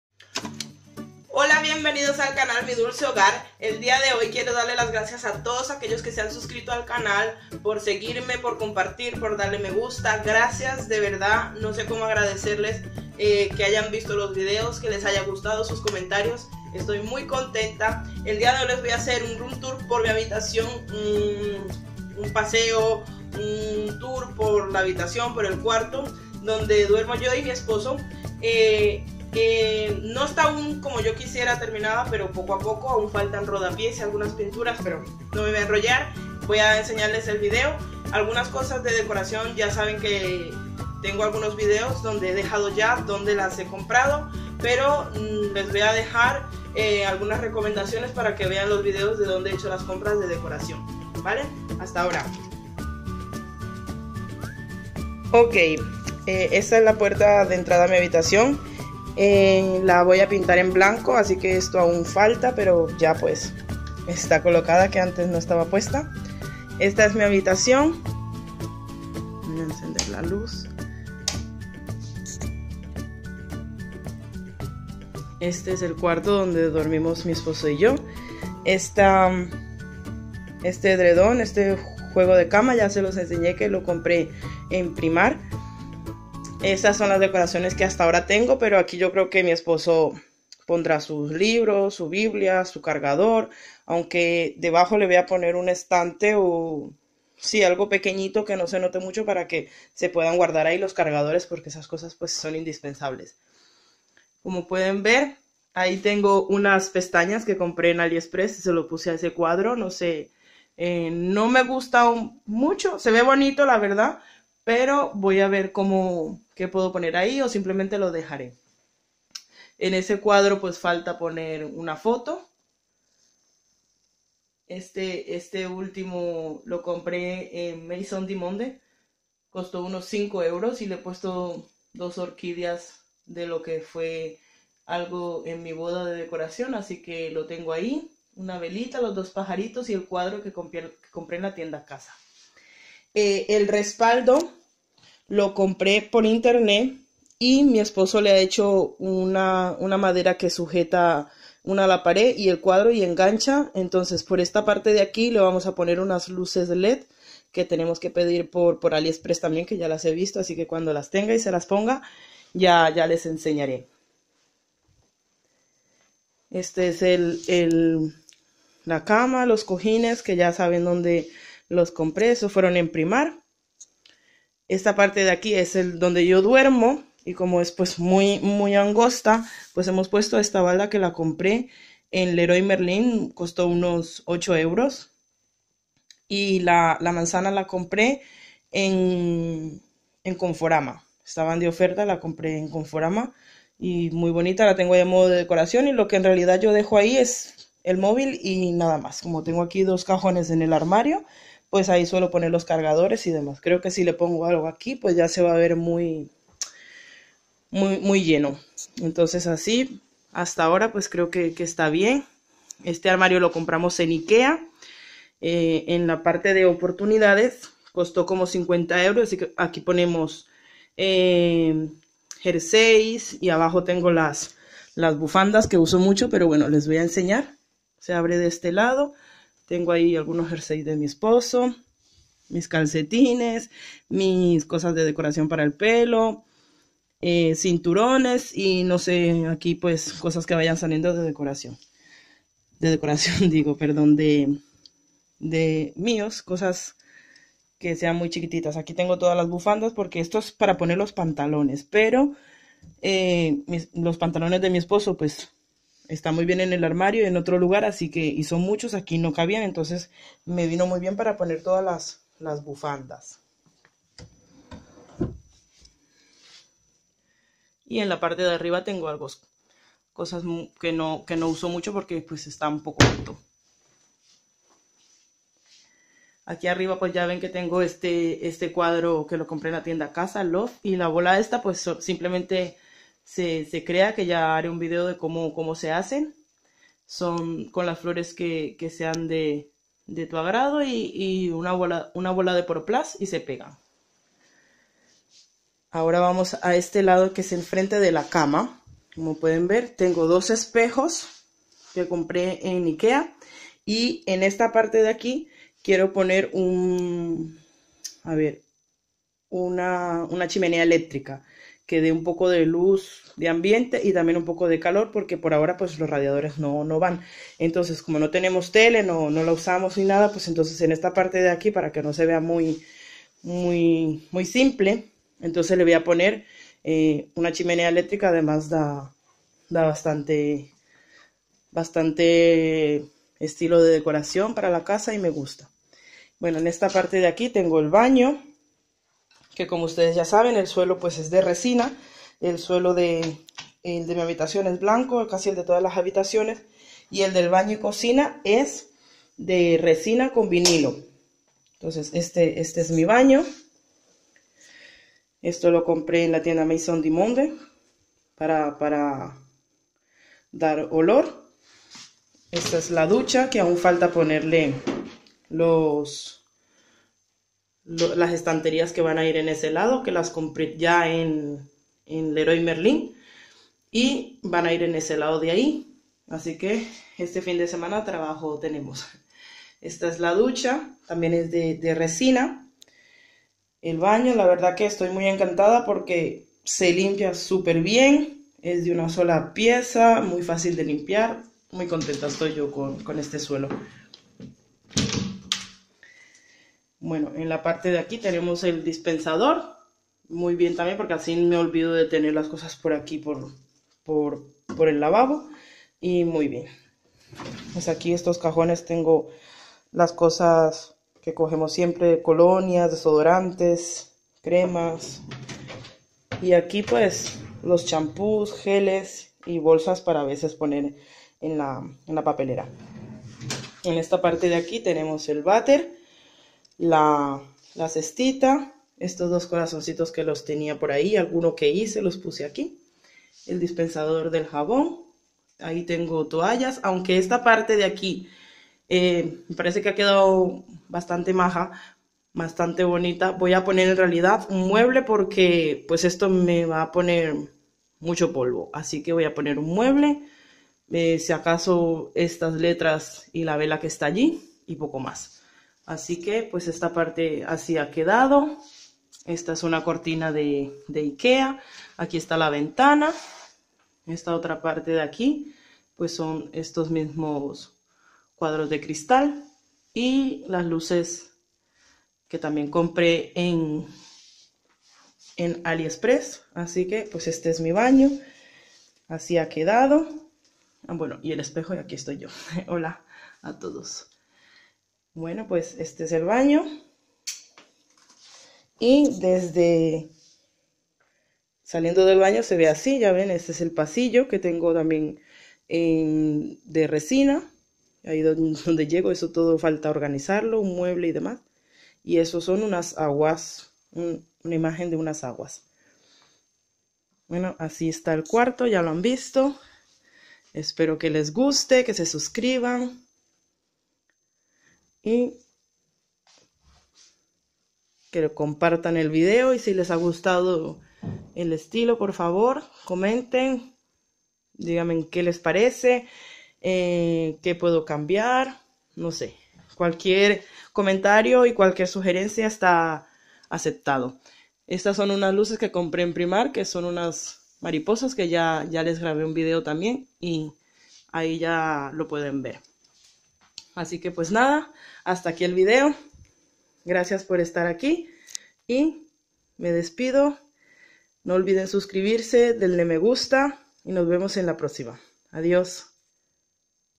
ROOM TOUR!!! 😍😘 TOUR POR MI HABITACION// DECORAR TU HABITACION// ORDENAR TU HABITACION
Aun no está totalmente terminada la habitación pero la decoración si está más o menos como quiero. Espero que les guste, que yo les puede aportar alguna idea de decoración.